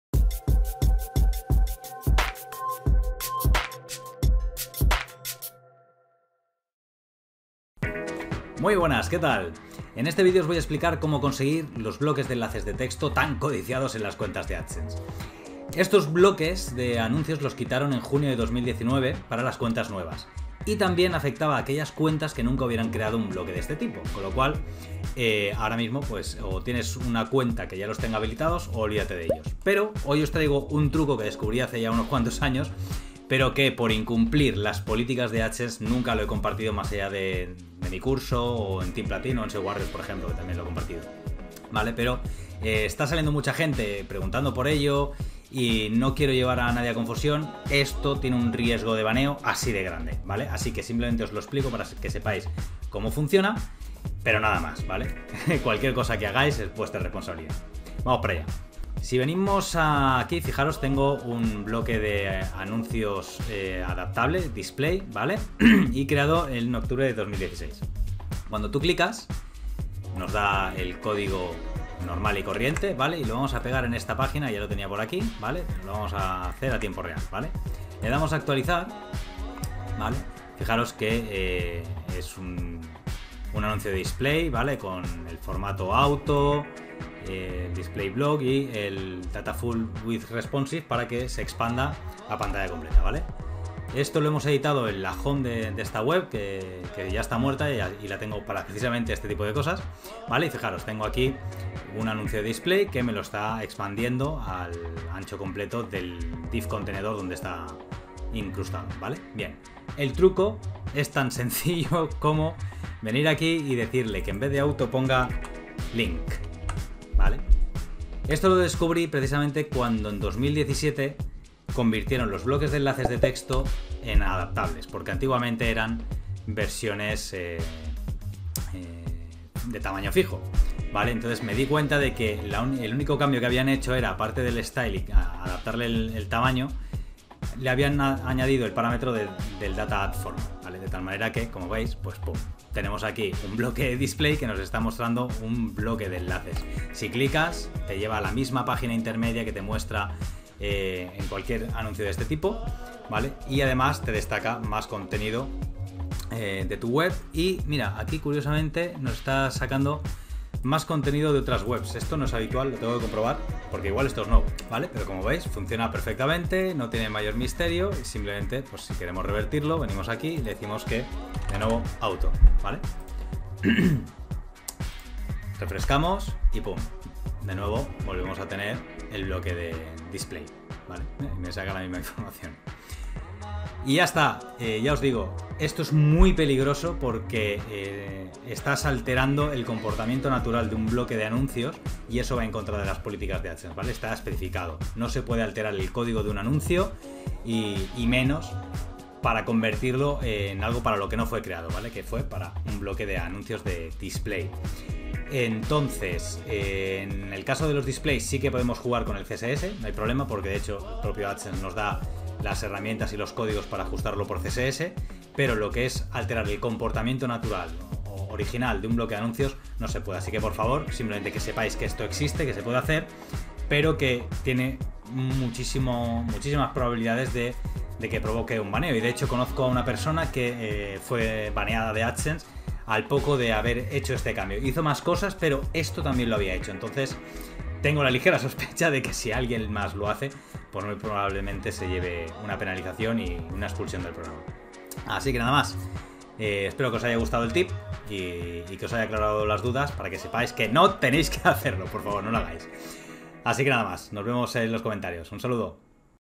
🔴 Cómo Crear Anuncios de Enlaces en Adsense [Si NO los Tienes] ☢️ ALTO RIESGO DE BANEO
¡MUCHO CUIDADO! Va en contra de las políticas de Adsense y os pueden CANCELAR la Cuenta. Más trucos de AdSense ➡️

Lo explico solo para que conozcáis el truco.

¿No tienes activos en tu cuenta de Adsense los bloques de anuncios de enlaces de texto? Hay un bug que permite activarlos en cualquier bloque adaptable de display, pero va en contra de las normas del programa.

Hazlo solo bajo tu cuenta y riesgo. No me hago responsable

Para activar los anuncios de enlaces de texto de Adsense solo tienes que  dale al play y ver cómo se hace en el vídeo ;)

Micro Snow Ball Ice
Webcam Logitech C920 HD Pro

✔️ ¡SÍGUEME! ✔️